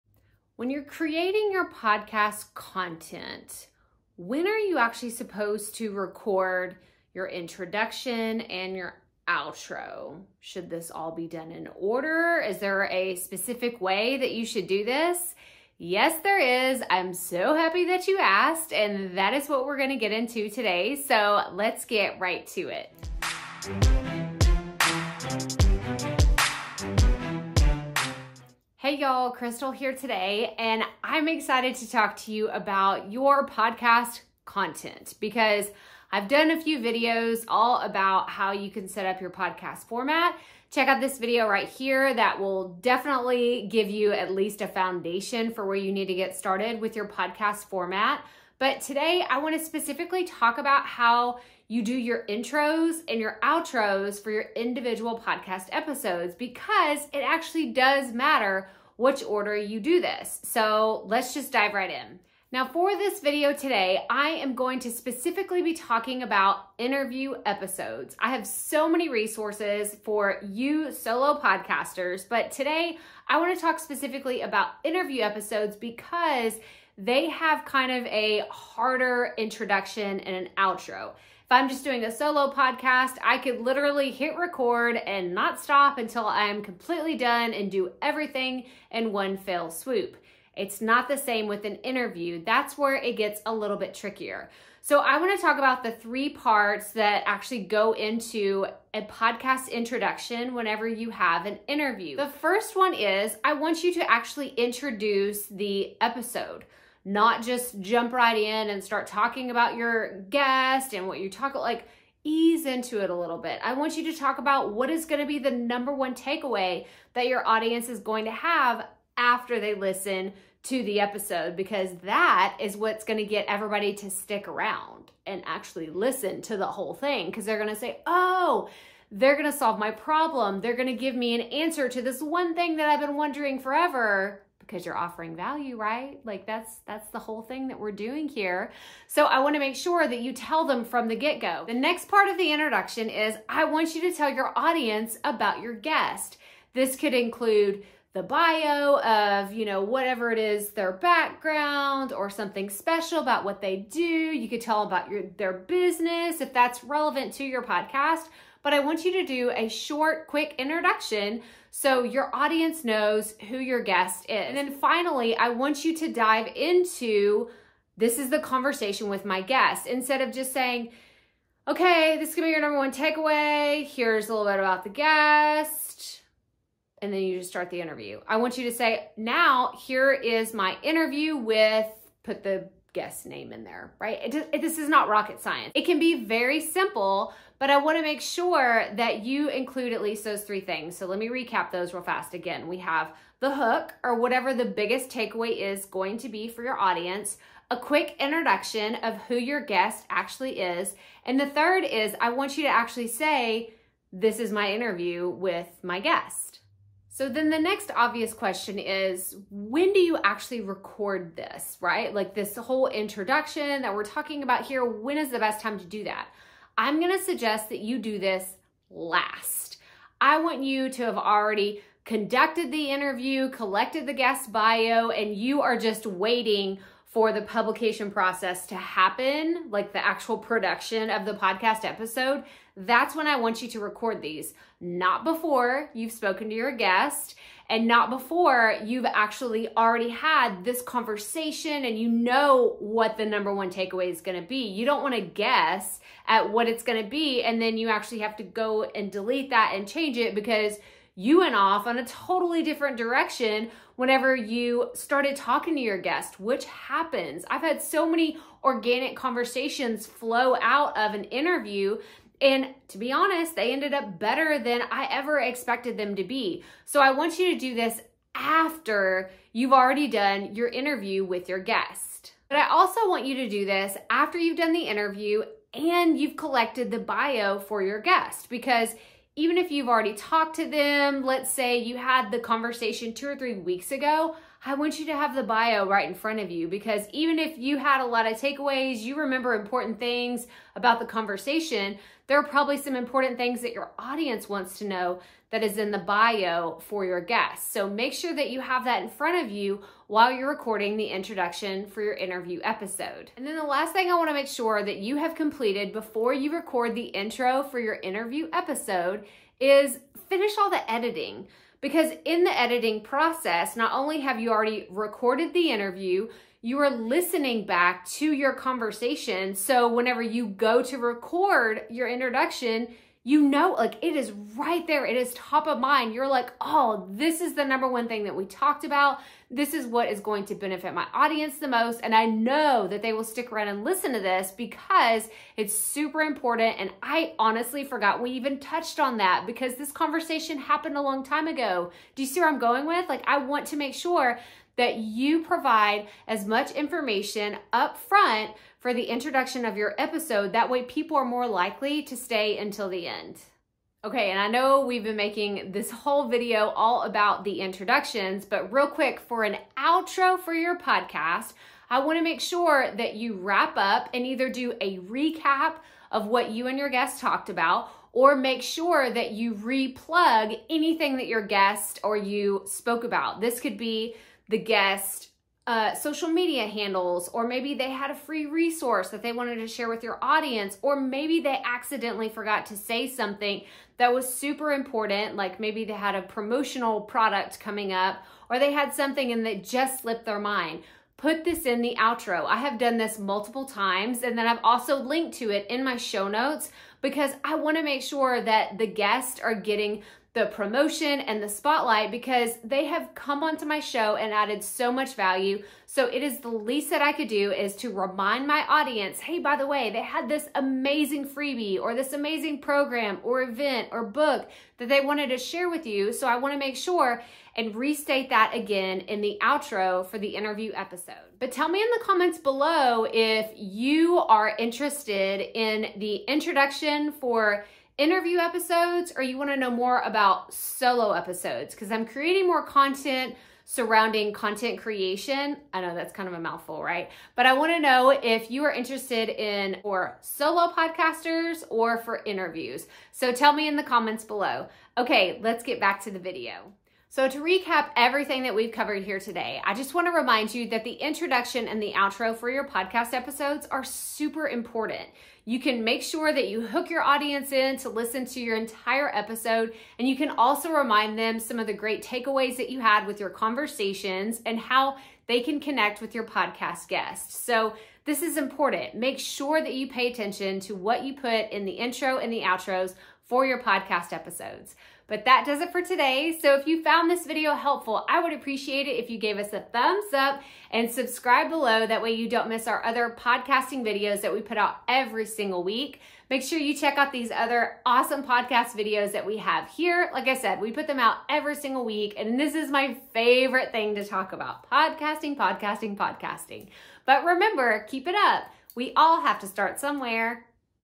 Best Podcast Intro and Outro Examples, Use These For Interviews!
How do you start the beginning of your podcast? And how are you supposed to end an episode? There are a few things to consider when you start recording and key points to make sure you hit on when you're producing an interview episode.

So, let's dive into how you can create your podcast intros and outros.

Check Out the FREE Podcast Training:
"From Struggling to Award-Winning: 4 Insider Secrets Every Podcaster Needs to Know"